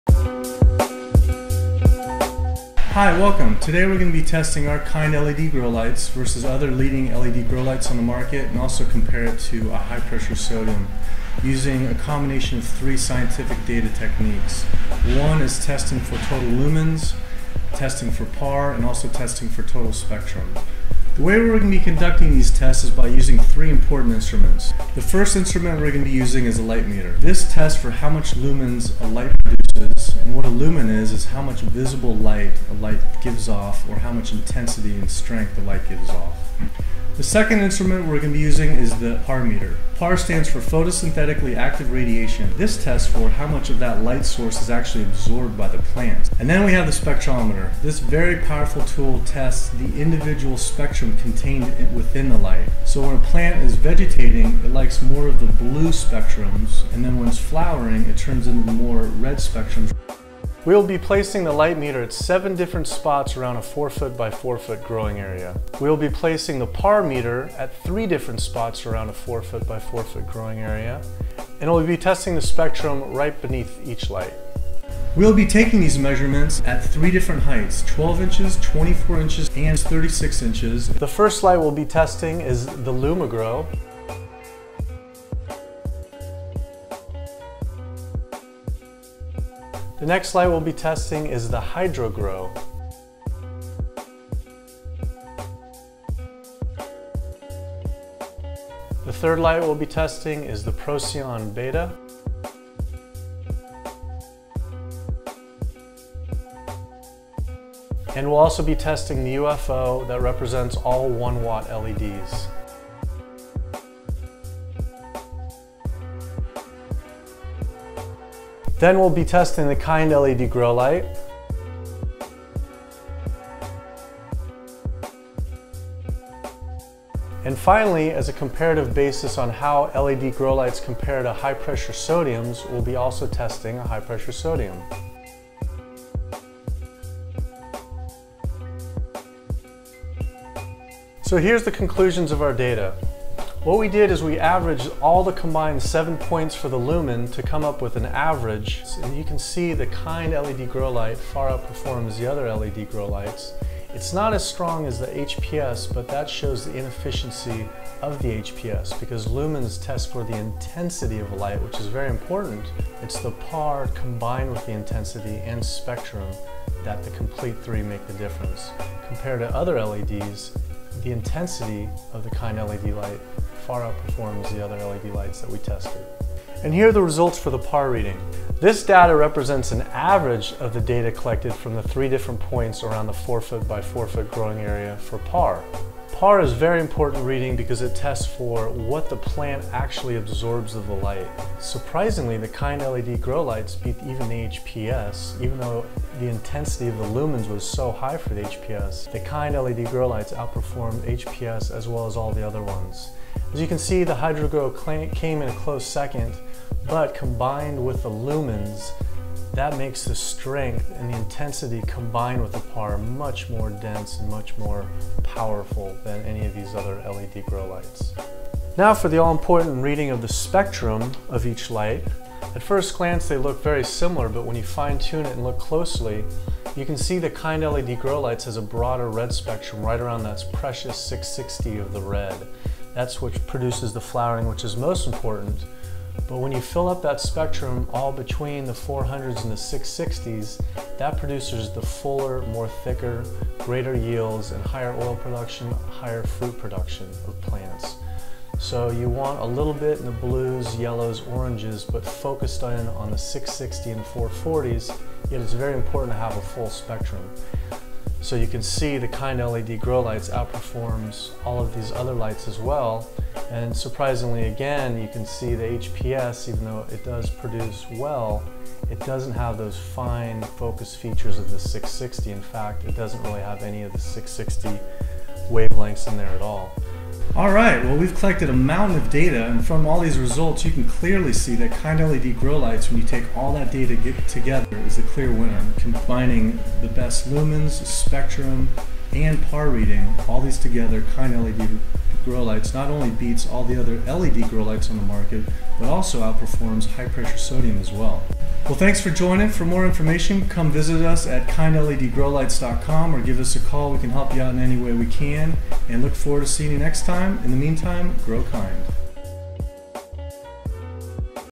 LED Grow Light Testing GrowLight Versus a 400 & 600 Watt HPS Kind LED Grow Lights Lamps LED Garden
Here is a very cool test we completed for you on the Kind LED. We tested many brands of LED's and this was the only one worth a write up.